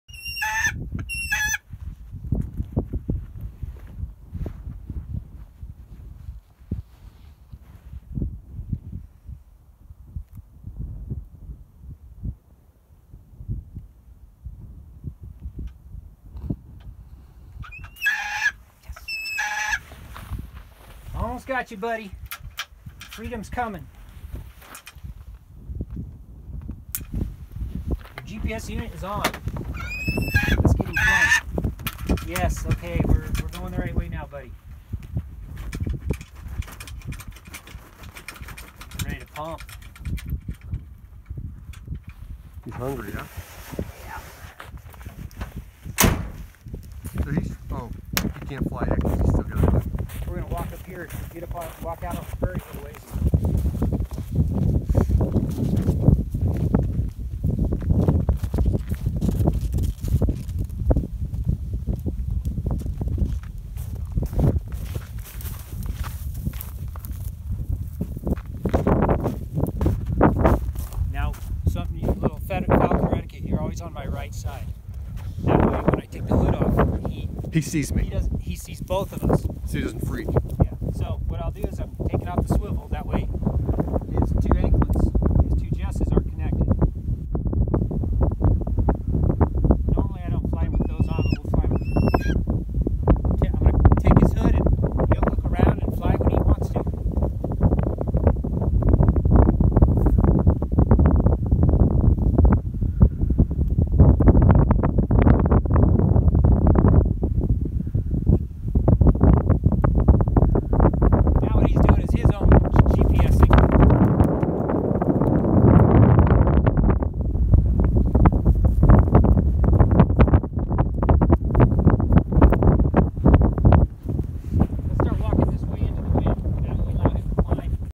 Hallux GPS Falcon Tracking Device
In this video, we show the easy placement of our Hallux GPS on a falcon and the Falcon leaving the glove.